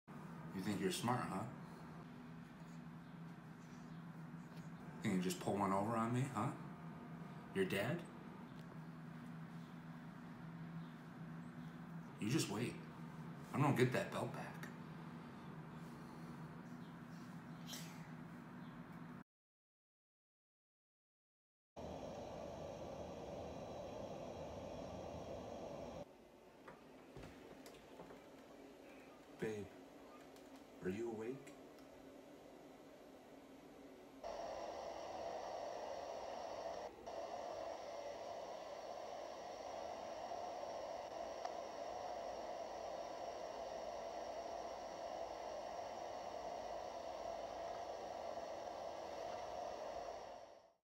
ICW Dippin Donuts 24/7 Title Change!
New ICW Dippin Donuts 24/7 Champion Crowned!